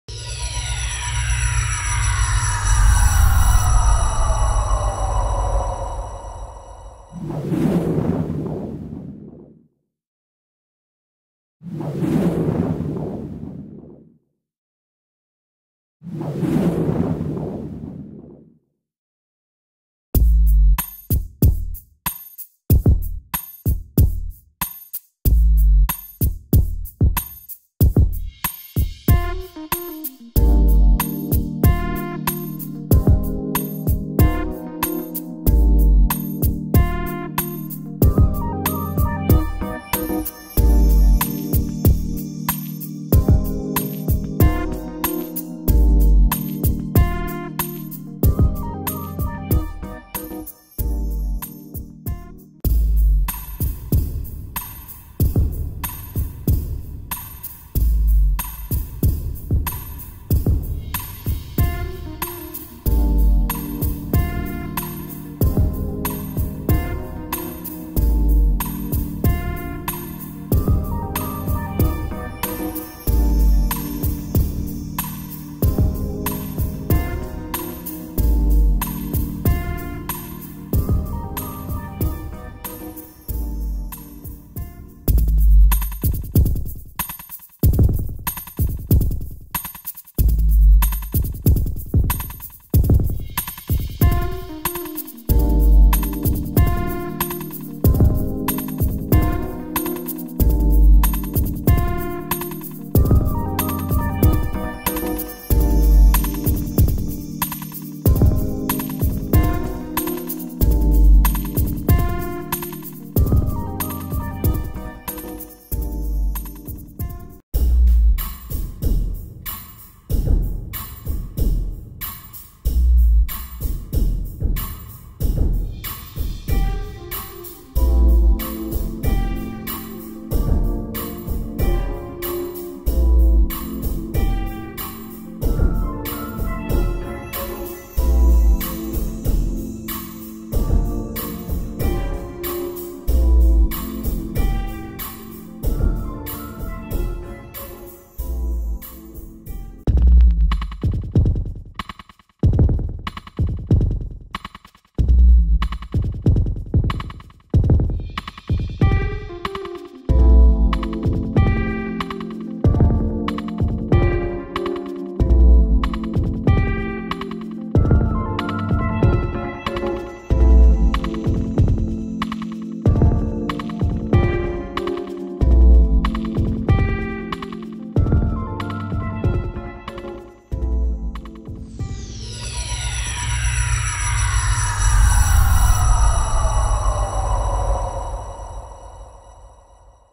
SwirlS
Fine Art CGI with animation, a colorful visionary journey, surreal graphic Op Art, a meditation